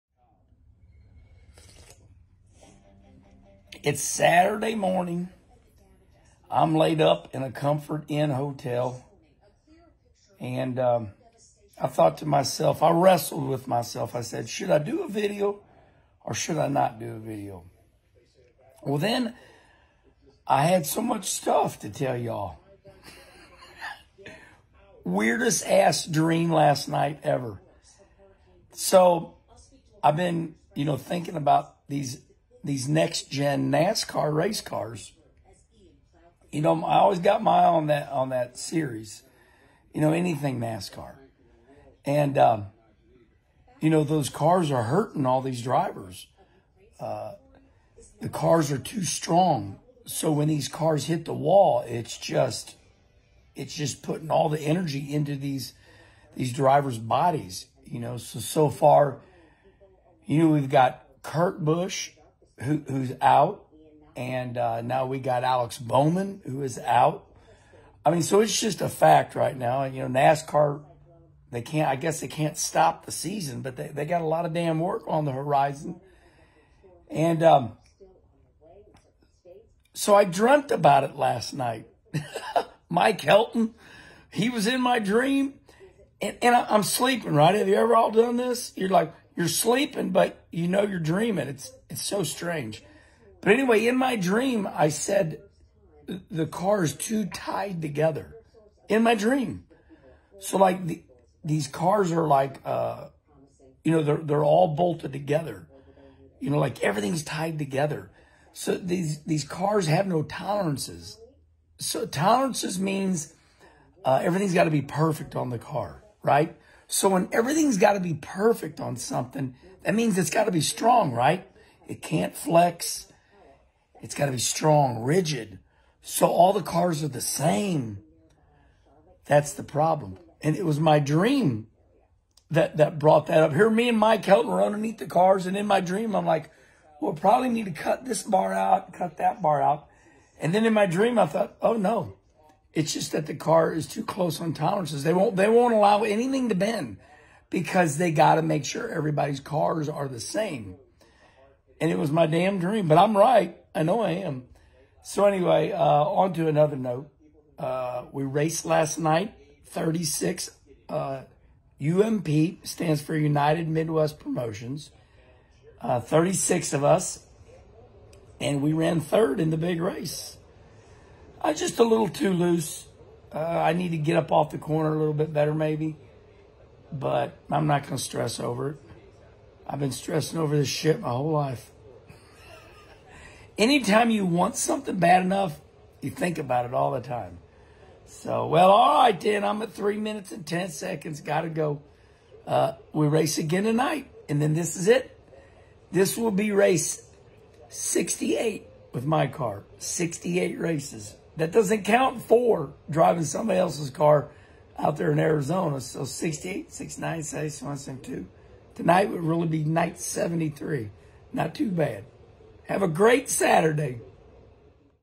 Let's Talk About Concussions & the Next Gen Car
Kenny Wallace wants to discuss NASCAR drivers suffering concussions in the Next Gen Car.